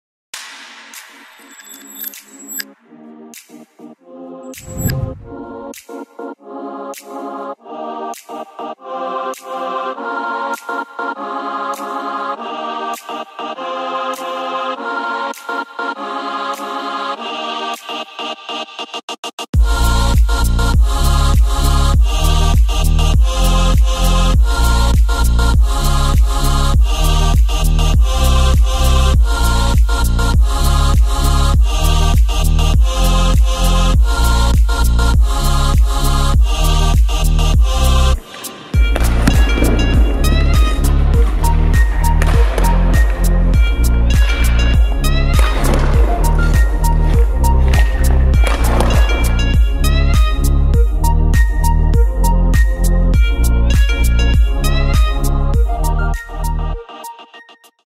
Kivikovat maalivahdit - Perusasento
Kesän jälkeen on hyvä kertailla asioita. Tsekkaa video ja laita peliasento minttiin!